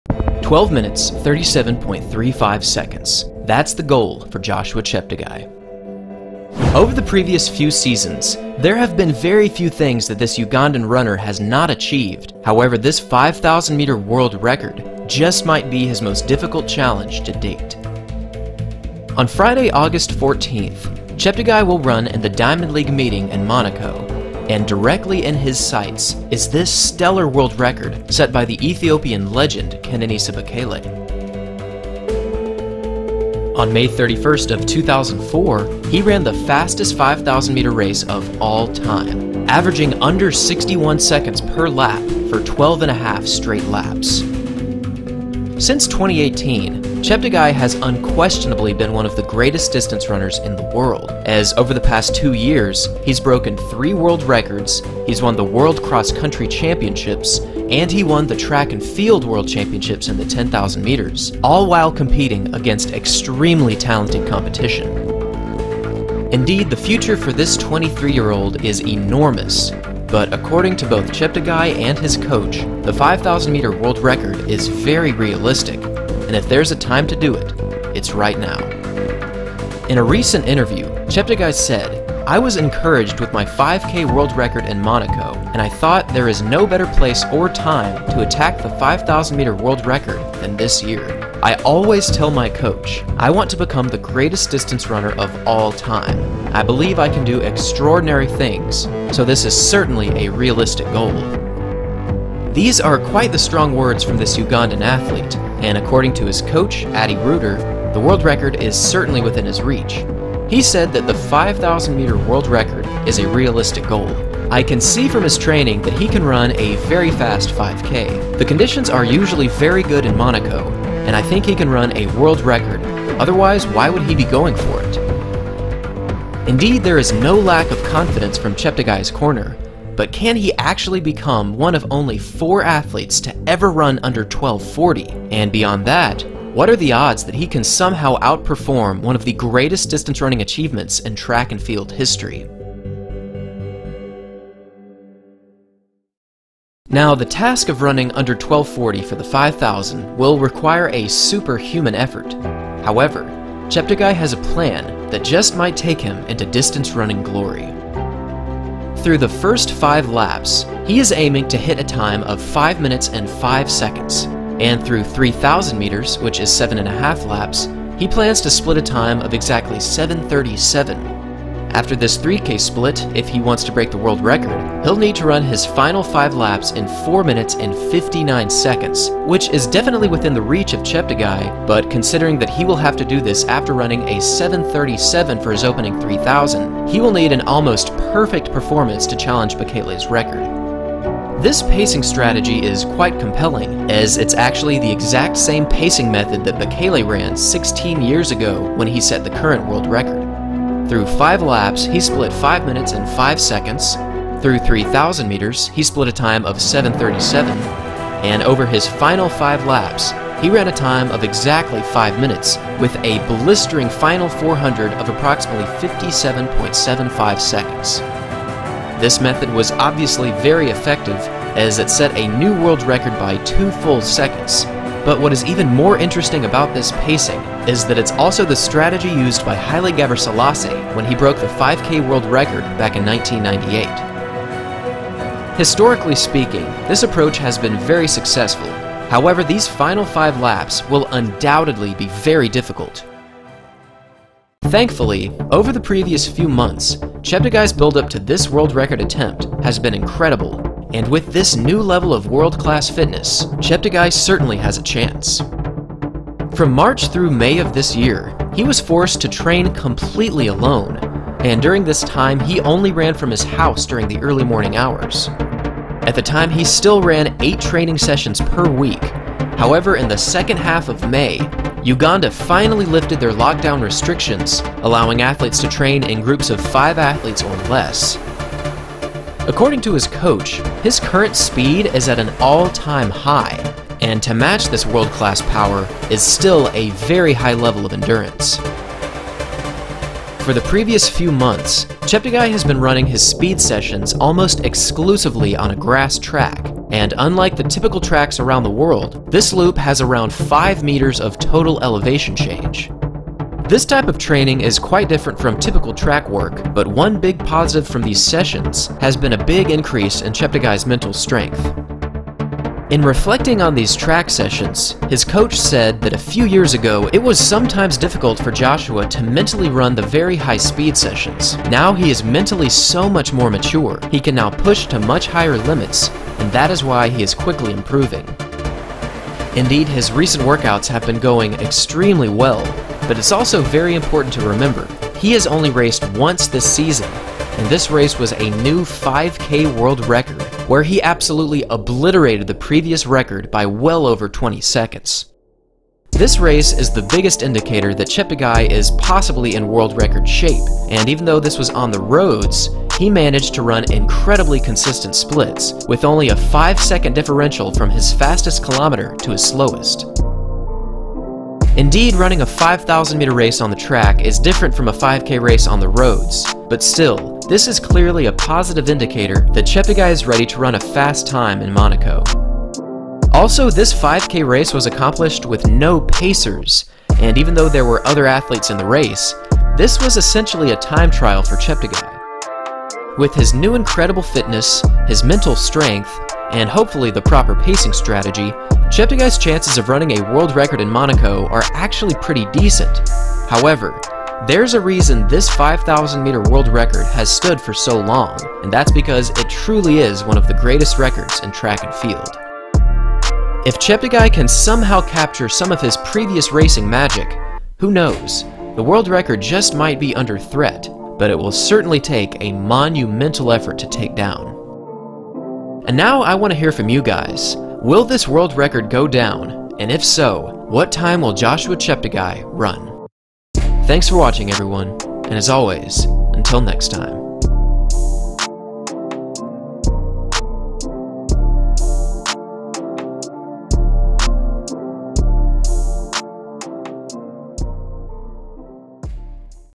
Joshua Cheptegei's Plan to Break the 5,000 Meter WORLD RECORD!!
Joshua Cheptegei is without doubt one of the most talented distance runners in the world.  Over the past 2 years, he's broken 3 world record, he's won 2 world championships, and now, he's chasing after his 4th world record.

At 12:37.35, Kenenisa Bekele's 5,000 meter world record has now stood for over 16 years, so the question now, is can Joshua Cheptegei run faster?